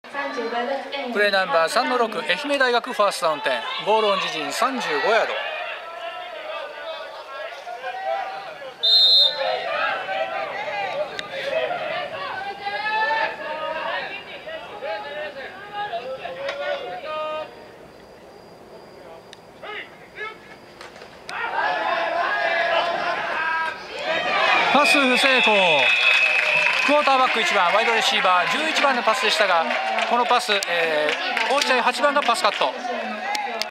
20181021 1070 Kochi OLB18 PassCut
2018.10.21 中四国学生アメリカンフットボール at 広島広域第二運動公園
優勝決定戦　高知大学MarineCorps × 愛媛大学Bombers